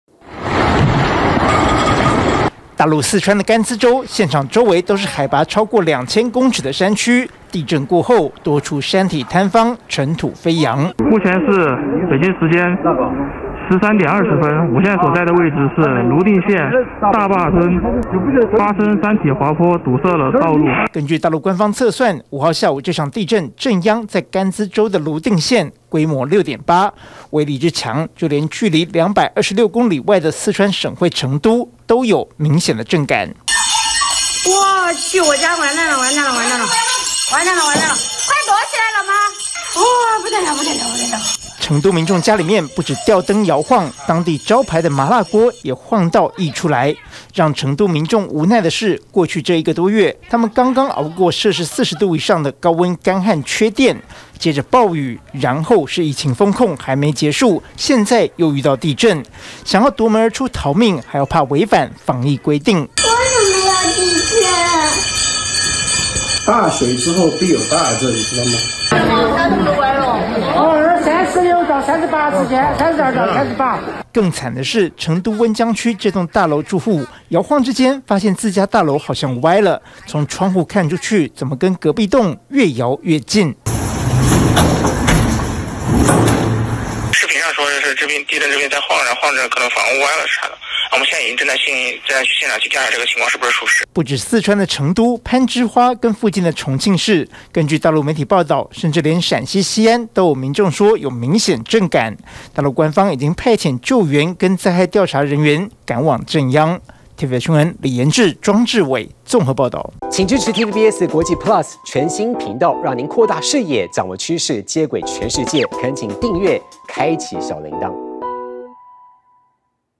陸四川甘孜規模6.8強震 釀7死 成都有感｜TVBS新聞
大陸四川甘孜州瀘定縣，今天下午發生規模6.8地震，包括正在全市居家隔離的成都和重慶，都有民眾表示感受到劇烈搖晃，不幸的是，目前當地官方通報，地震已經造成7人死亡。